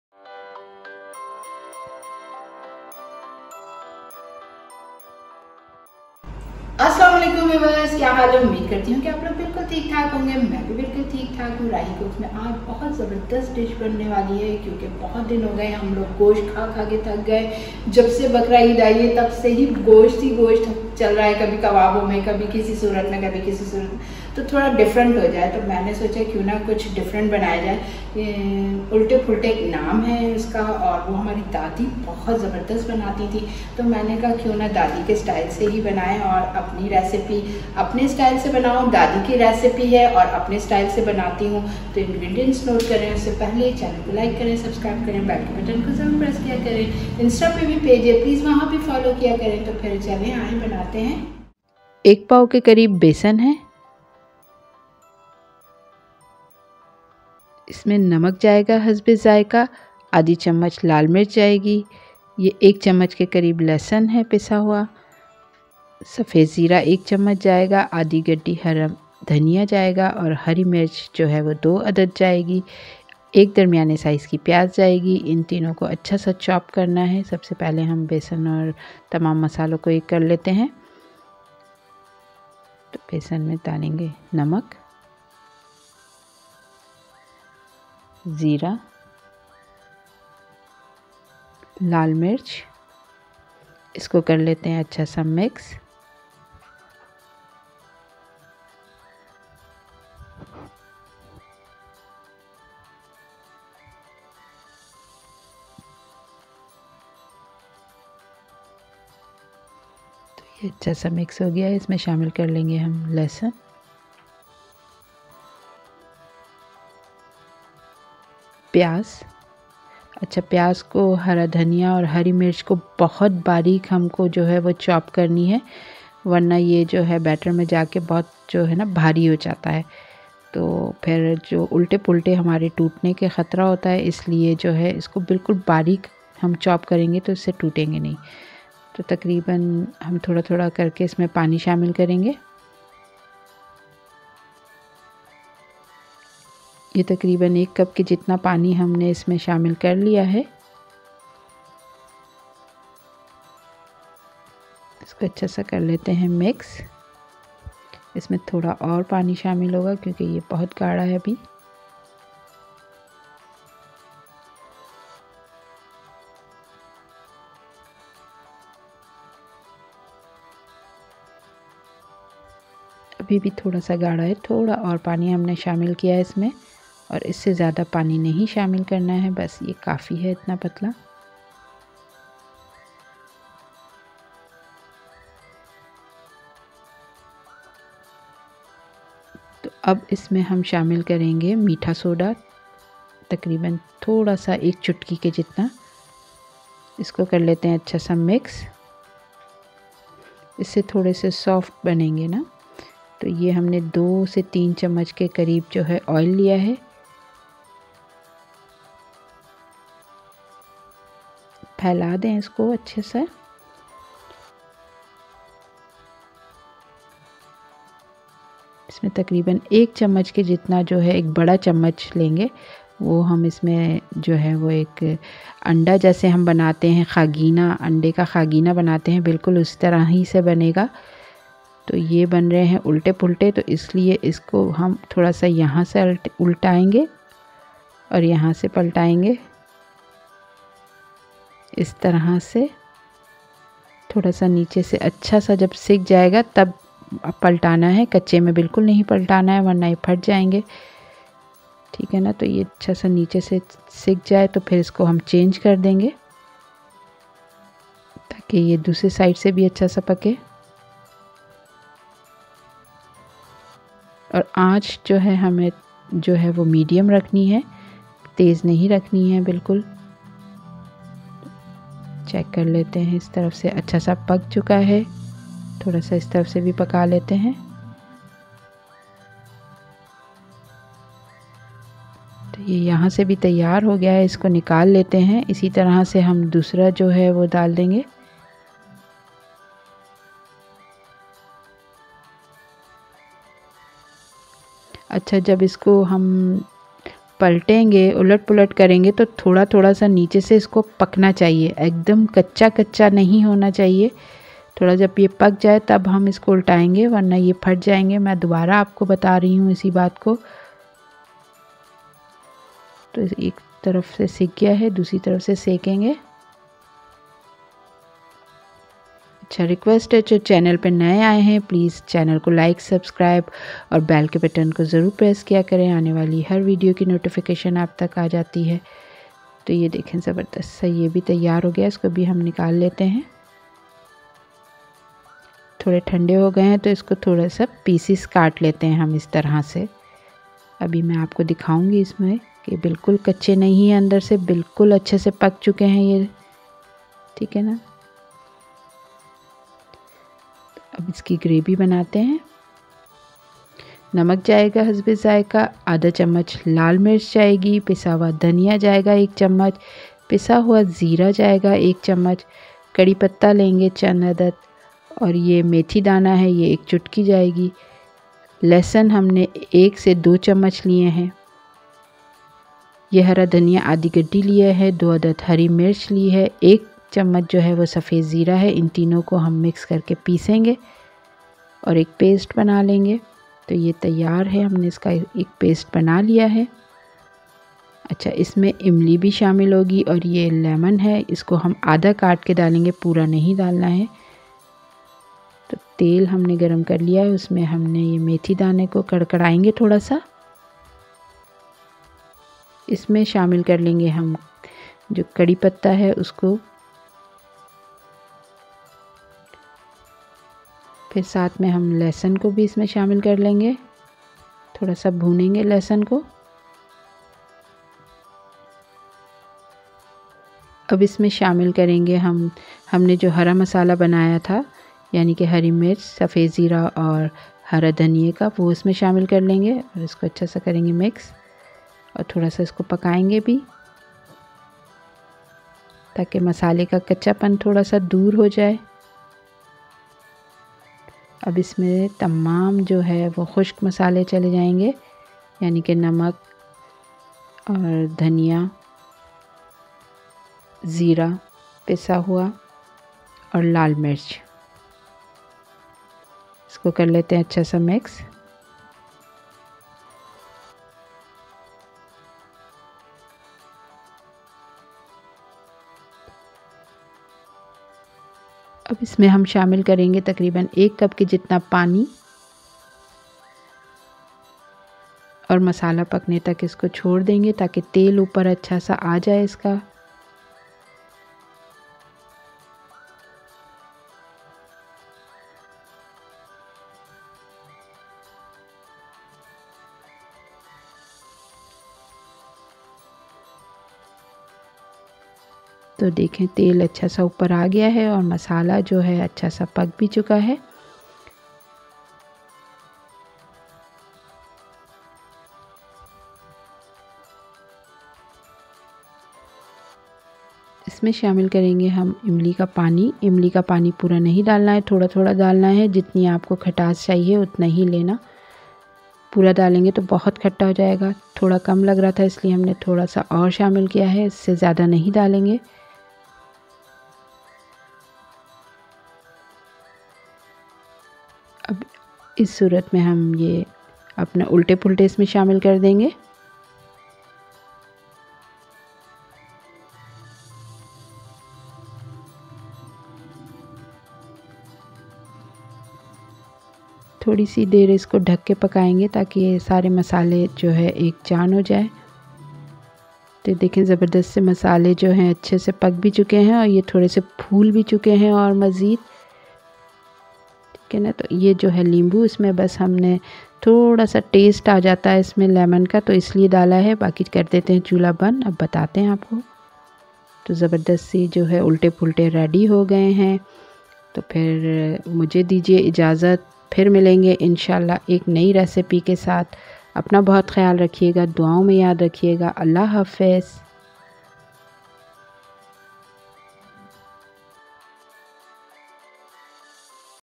Altay Paltay Recipe | Besan k Altay Paltay Recipe by Rahi Cooks
INGREDIENTS:
•Gram Flour 250 gms
•Coriander leaves ½
For Gravy
•Curry leaf 5-6
•Fenugreek seeds 1 (pinch)
•Coriander leaves ½
•Cumin seeds 1 Tbsp
•Tamarind 1 cup
•Lemon ½
•Cooking oil